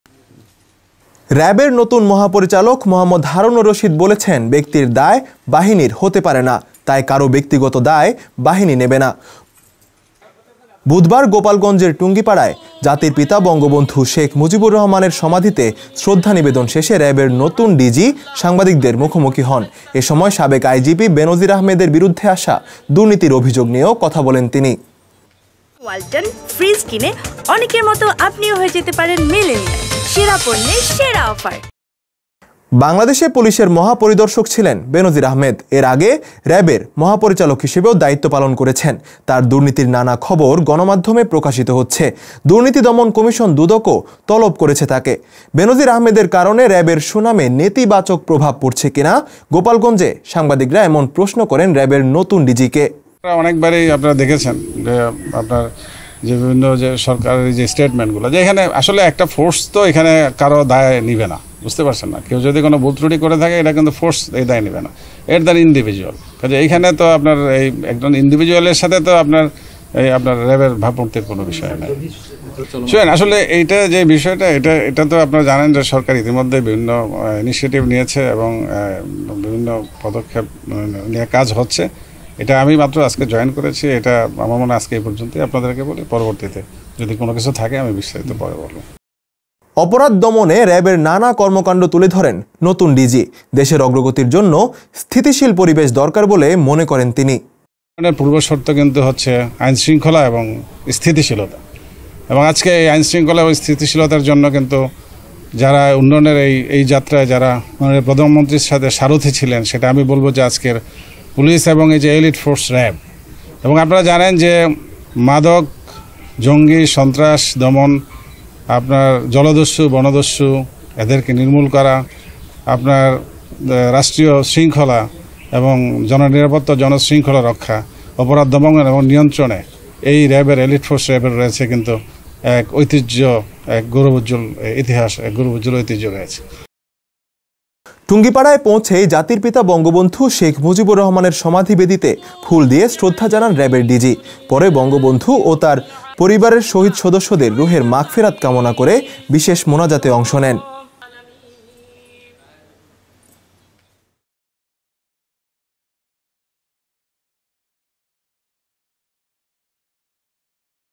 সাবেক ডিজি বেনজীরকে নিয়ে কথা বলেছেন র‍্যাবের নতুন ডিজি
র‍্যাবের নতুন ডিজি মো. হারুন অর রশিদ বলেছেন, ব্যক্তির দায় বাহিনীর হতে পারে না।